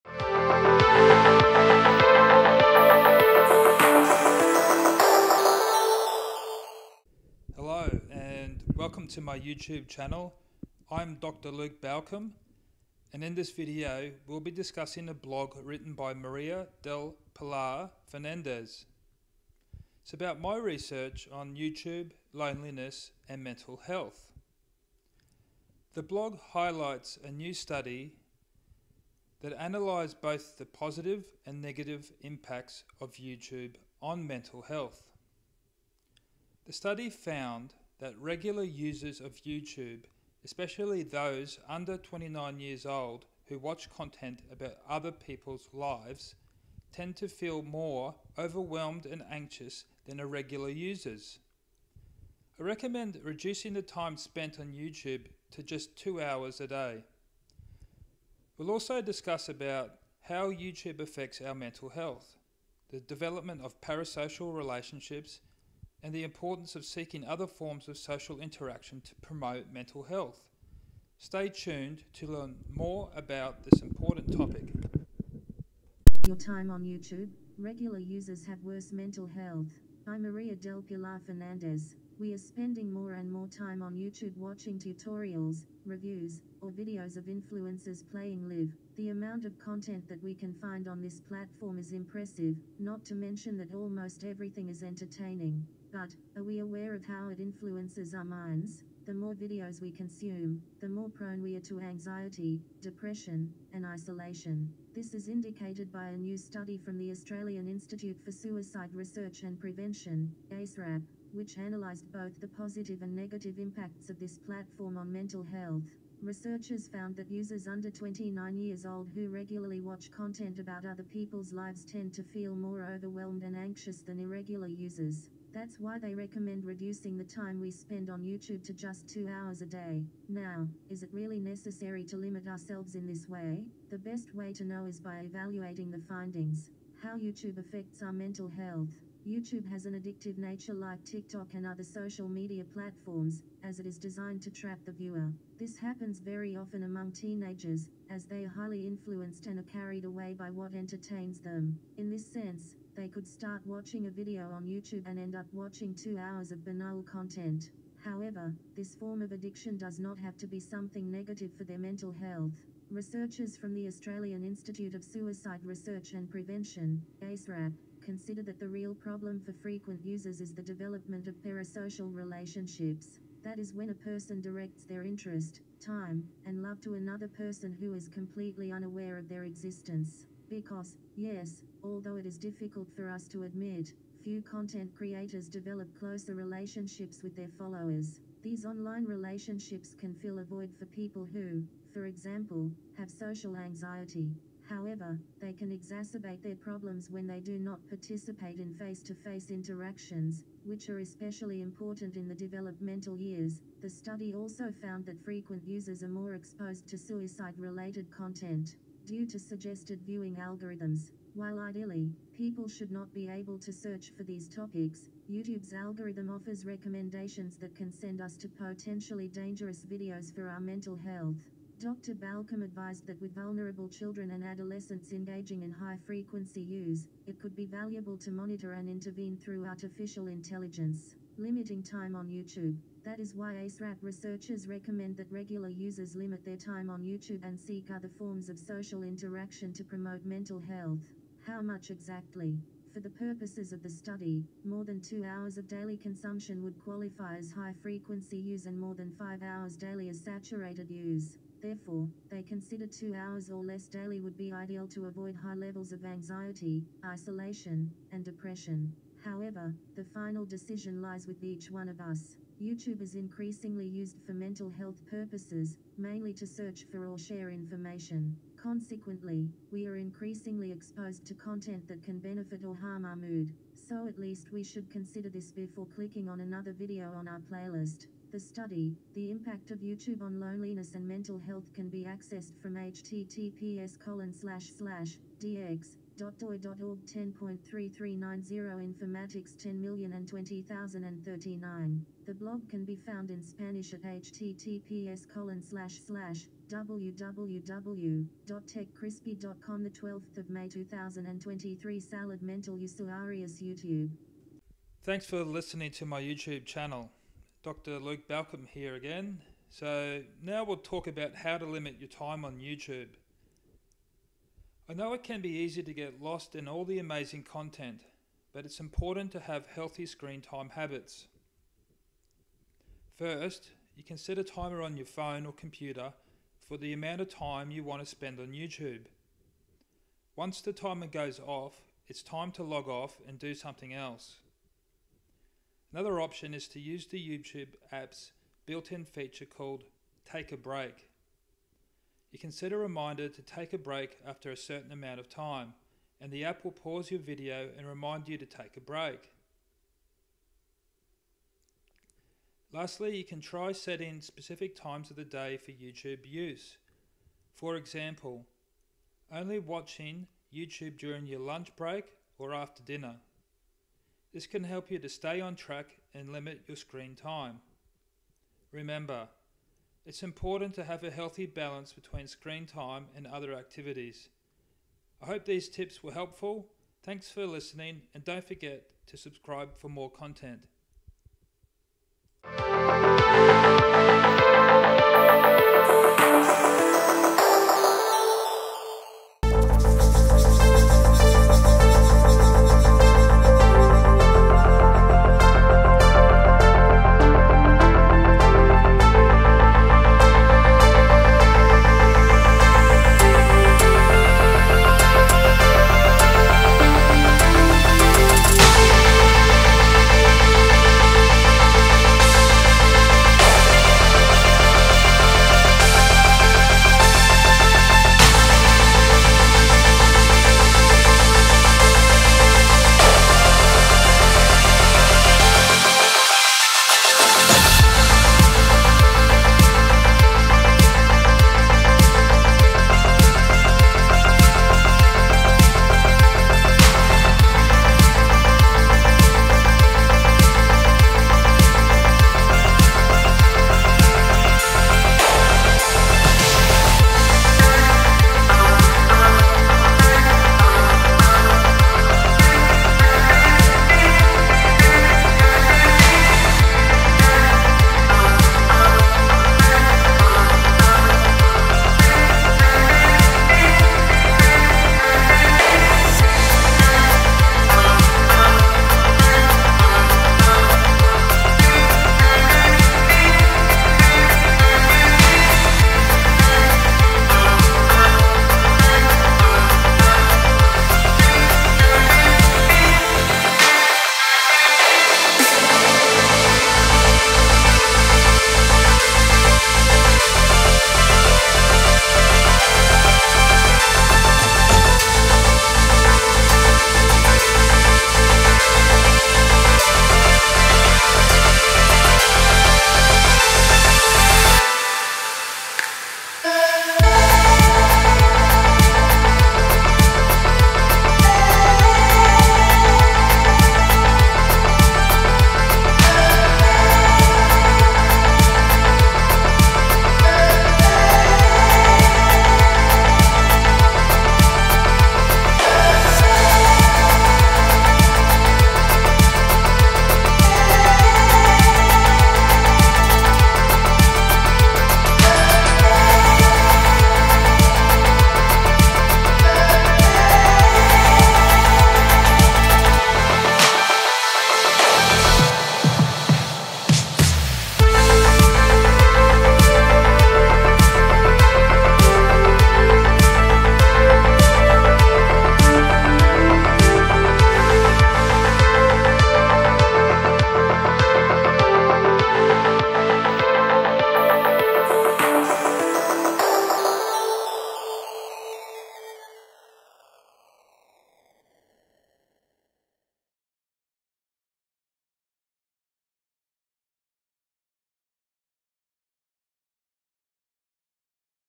YouTube and mental health: Vlog 1
The first vlog on 'YouTube and mental health'. Today, we talk about how YouTube effects our mental health and why we should be limiting time on here. Dr Luke Balcombe introduces a blog by Maria del Pilar Fernandez translated from Spanish, and follows up with some tips on taking a break from YouTube.